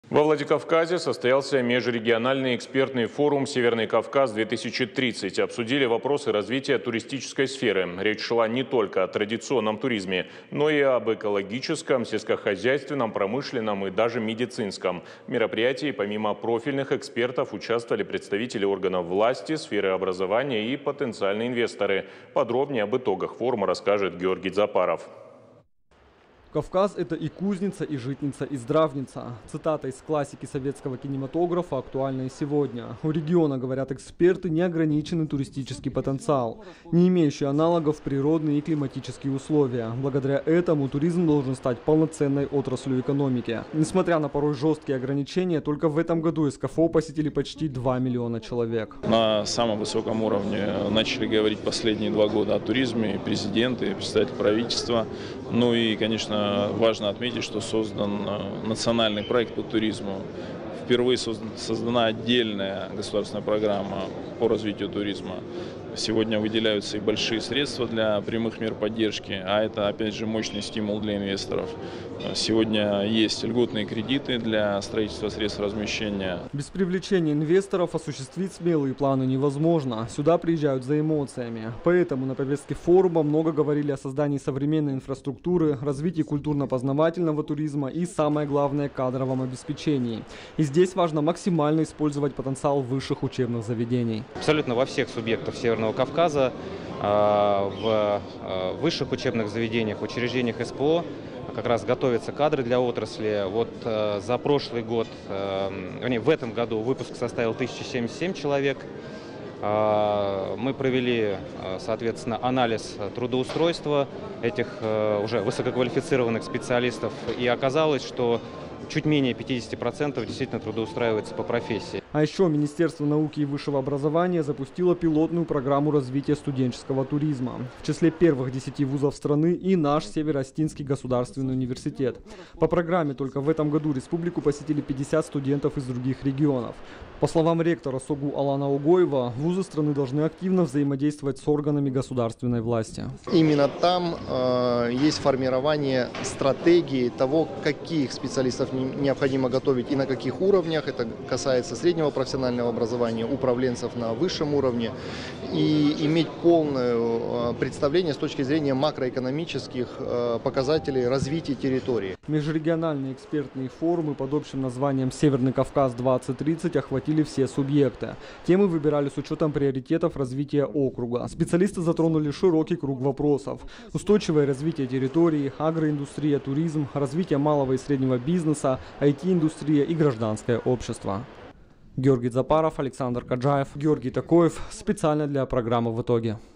Во Владикавказе состоялся межрегиональный экспертный форум «Северный Кавказ-2030»
Во Владикавказе состоялся межрегиональный экспертный форум «Северный Кавказ-2030». Обсудили вопросы развития туристической сферы. Речь шла не только о традиционном туризме, но и об экологическом, сельскохозяйственном, промышленном и даже медицинском. В мероприятии помимо профильных экспертов участвовали представители органов власти, сферы образования и потенциальные инвесторы.

Кавказ — это и кузница, и житница, и здравница. Цитата из классики советского кинематографа актуальна и сегодня. У региона, говорят эксперты, неограниченный туристический потенциал. Не имеющие аналогов природные и климатические условия. Благодаря этому туризм должен стать полноценной отраслью экономики. Несмотря на порой жесткие ограничения только в 2021 году СКФО посетили почти два миллиона человек.

Без привлечения инвесторов осуществить смелые планы - невозможно. Сюда приезжают за эмоциями. На повестке форума много говорили о создании современной инфраструктуры, развитии культурно – познавательного туризма и самое главное - кадровом обеспечении. И здесь важно максимально использовать потенциал высших учебных заведений.

А ещё министерство науки и высшего образования запустило пилотную программу развития студенческого туризма. В числе первых десяти вузов страны и наш Северо-Осетинский государственный университет. По программе только в 2021 году республику посетили пятьдесят студентов из других регионов. По словам ректора СОГУ Алана Огоева, вузы страны должны активно взаимодействовать с органами государственной власти.

Межрегиональные экспертные форумы под общим названием «Северный Кавказ – 2030» охватили все субъекты. Темы выбирали с учётом приоритетов развития округа. Специалисты затронули широкий круг вопросов: устойчивое развитие территории, агроиндустрия, туризм, развитие малого и среднего бизнеса, IT-индустрия и гражданское общество.
Мы рядом в:
ok.ru/irystontv